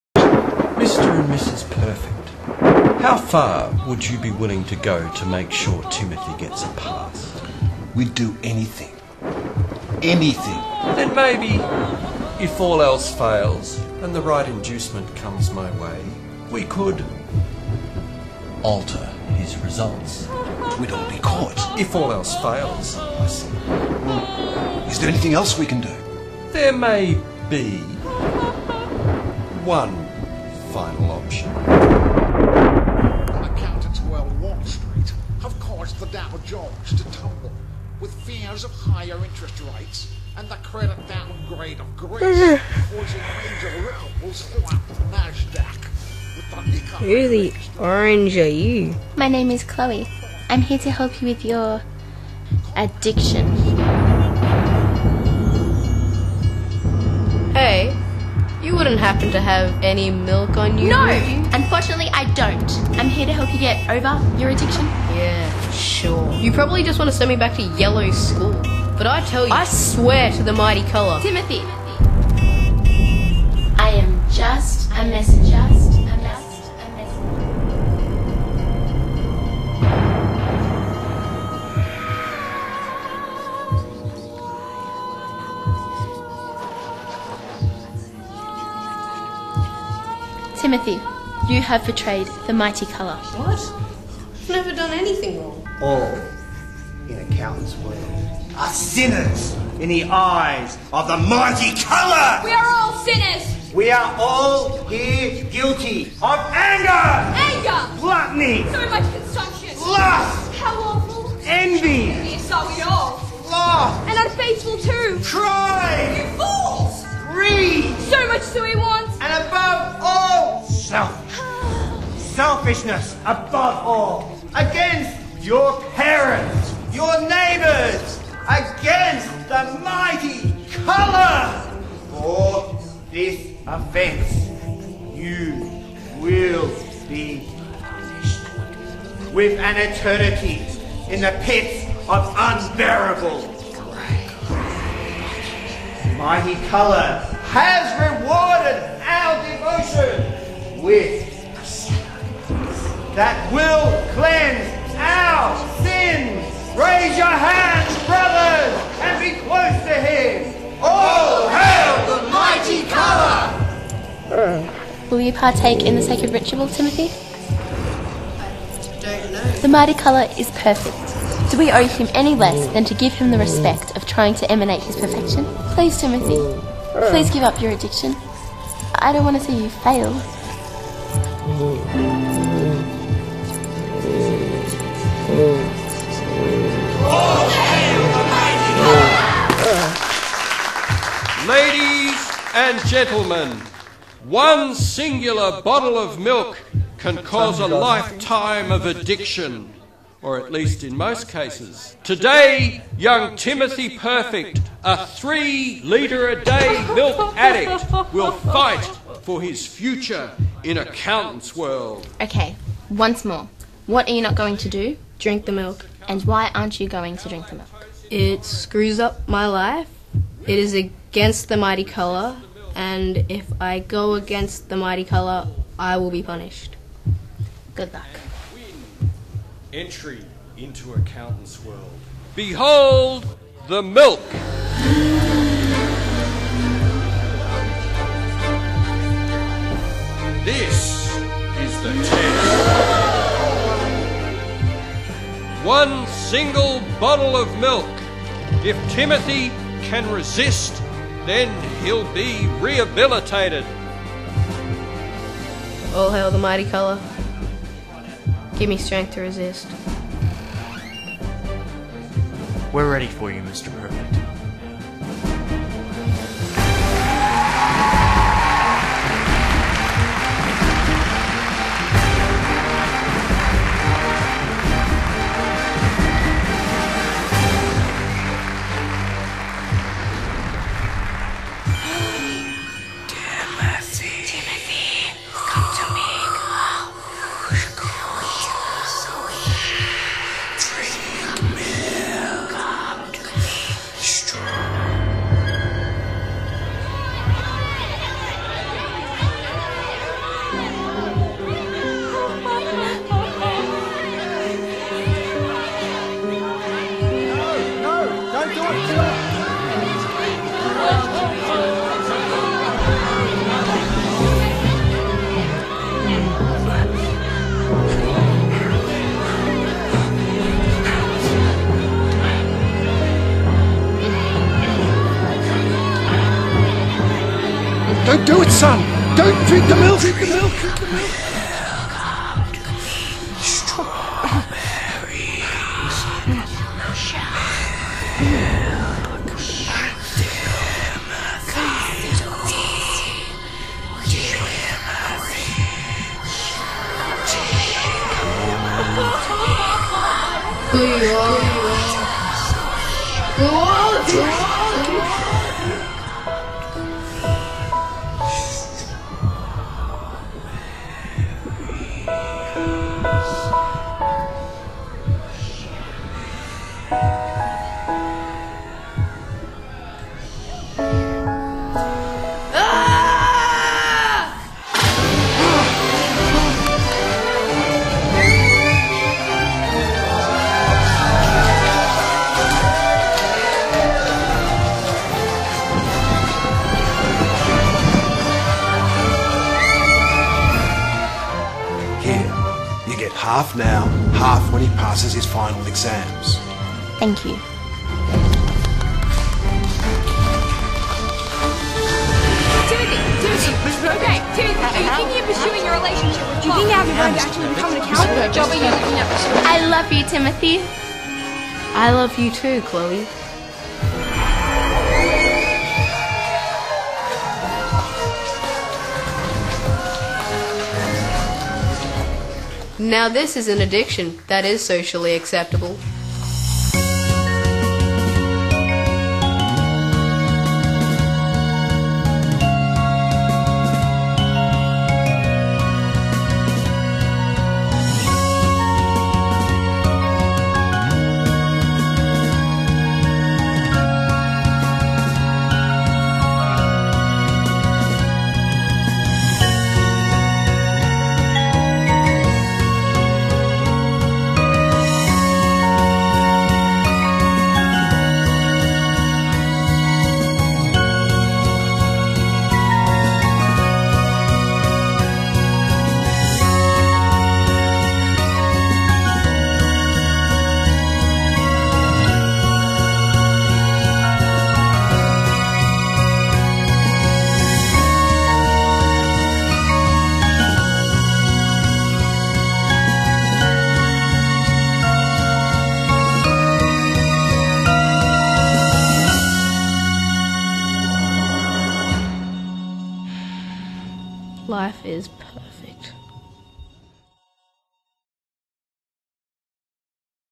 Accountant's World 3/3
The race is now on for Timothy's parents and his school to "cure" of him of his milk addiction in time for him to be able to sit his final exams. How far will they be willing to go? The final part - the final questions - the final test!!!